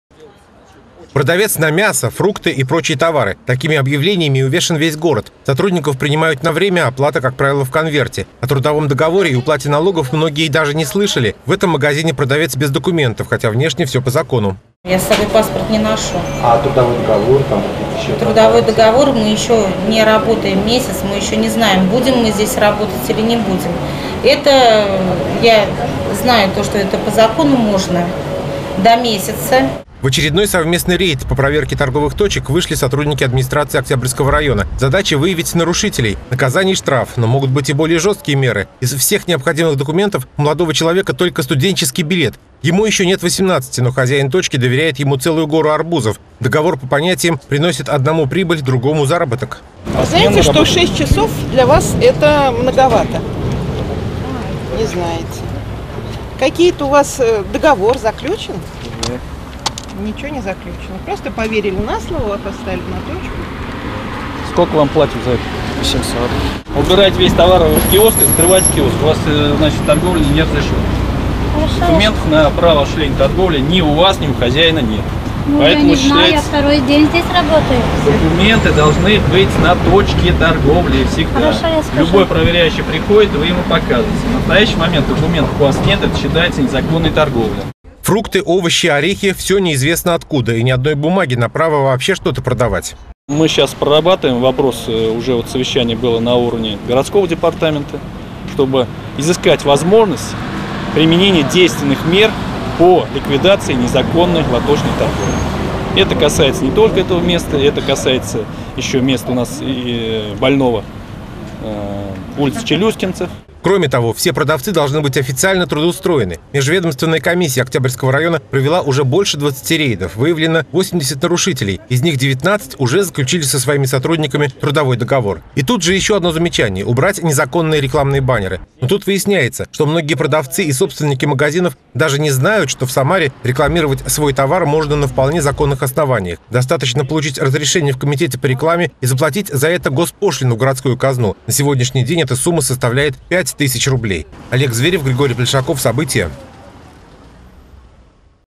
Городские власти наведут порядок в уличной торговле
Навести порядок в уличной торговле - городские власти ставят амбициозную задачу. Хотя и трудную сегодня  многие торговцы работают незаконно, а это значит что город терпит убытки: налоги от таких продаж не поступают в казну, а сами продавцы лишаются самых насущных социальных гарантий